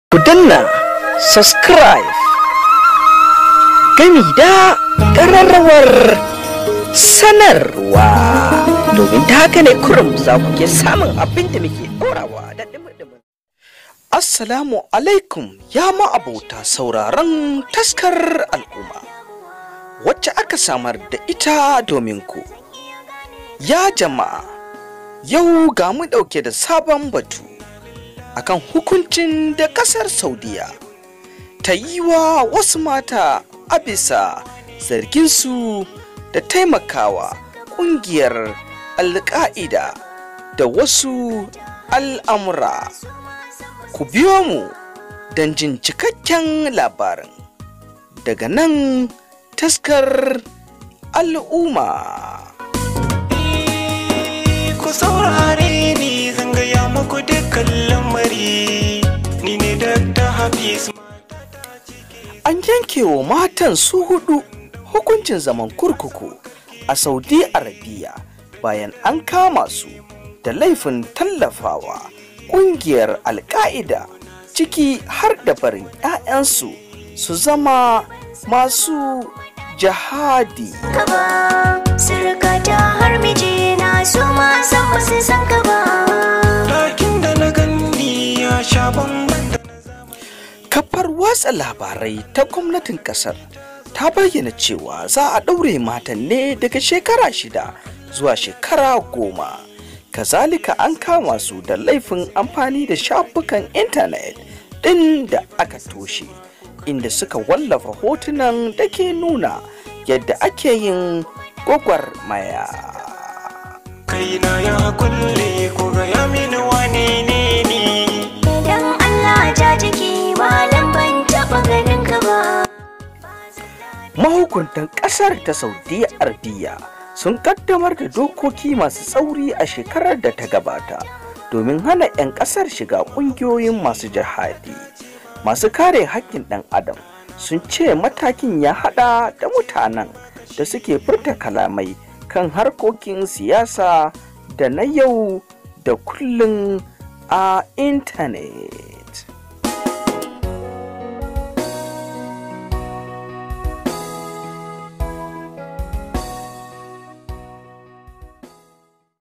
Kasar Saudia ta daure wasu mata 4 yan asalin kasar daurin shekara 10 agidan yari.
Ku niyomu danjin dalilin hakan